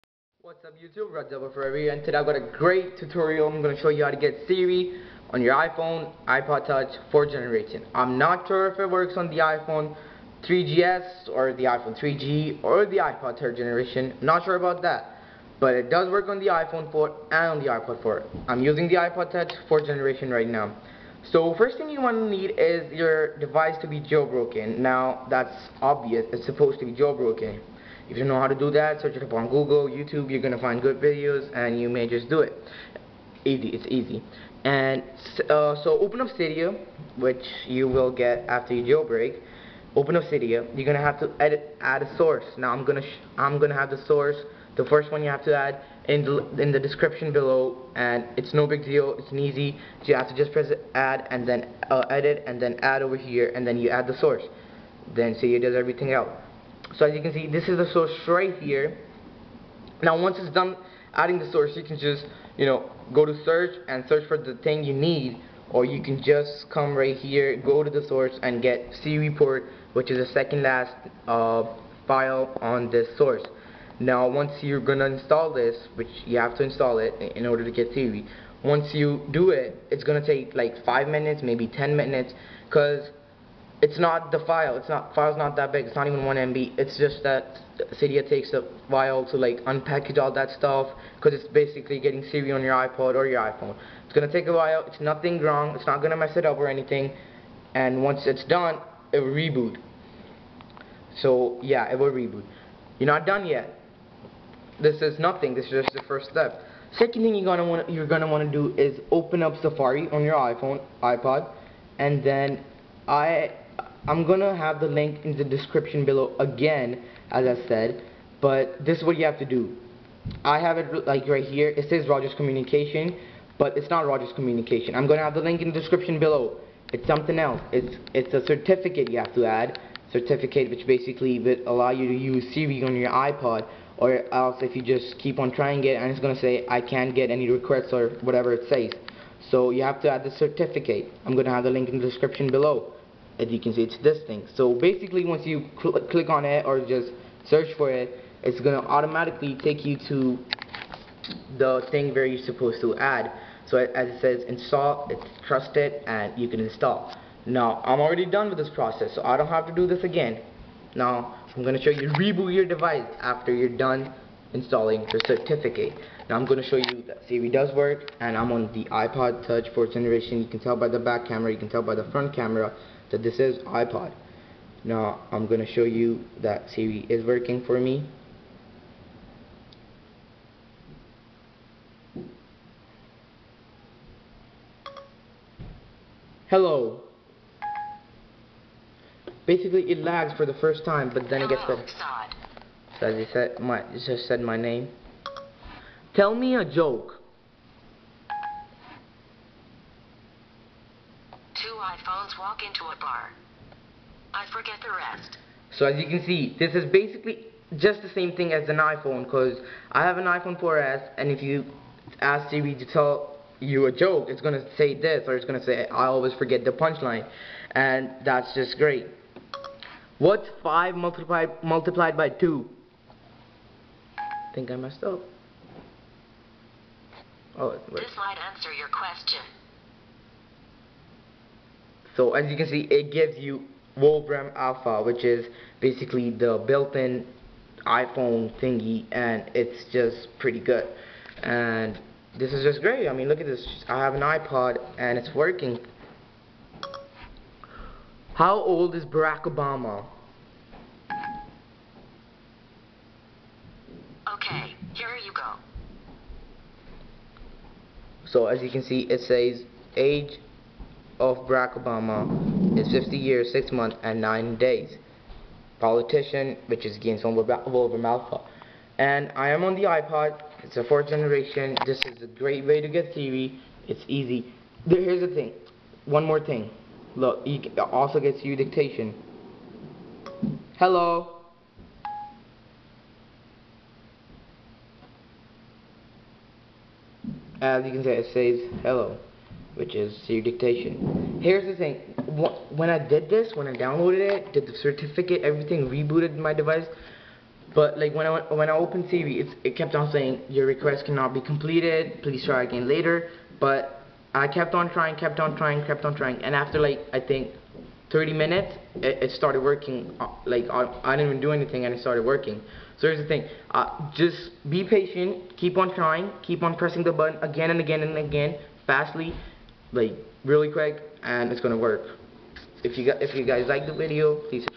How to install Siri on the iPhone 4 and iPod Touch using SiriPort
This tutorial shows you how to get Siri on your iPod touch or iPhone 4